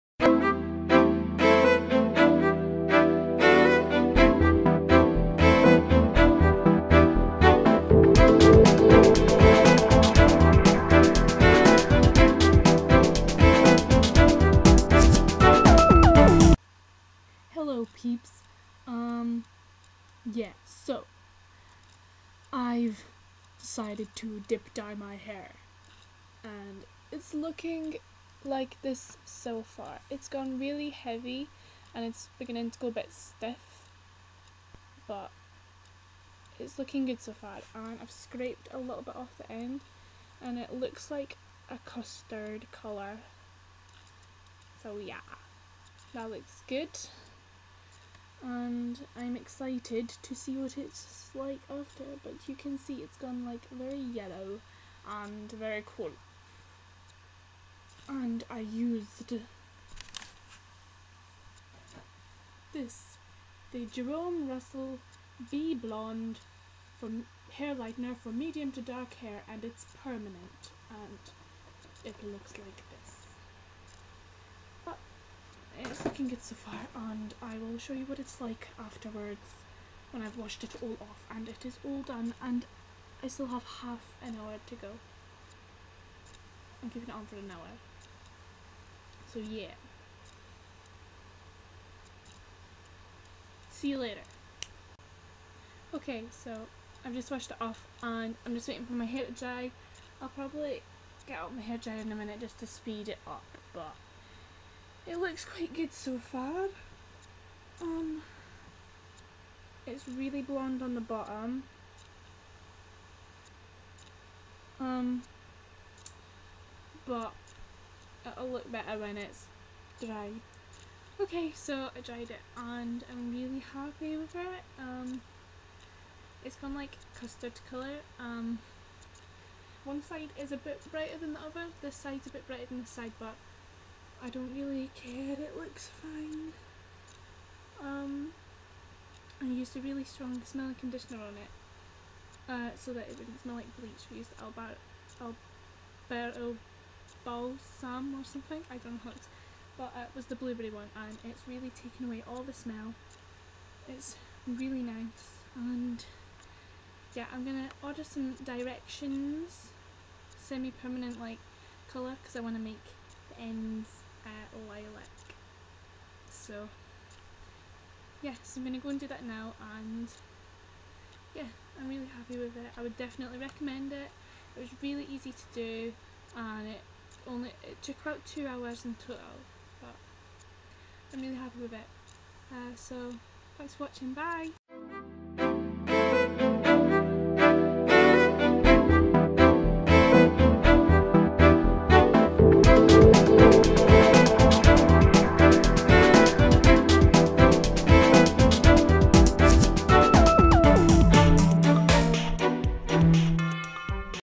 How I Dip-Dyed My Hair
Just a quick video from when I was dip-dying my hair a few dyas ago. It turned out great and would definatley use the product again. It was also really easy and simple to do and I love the results!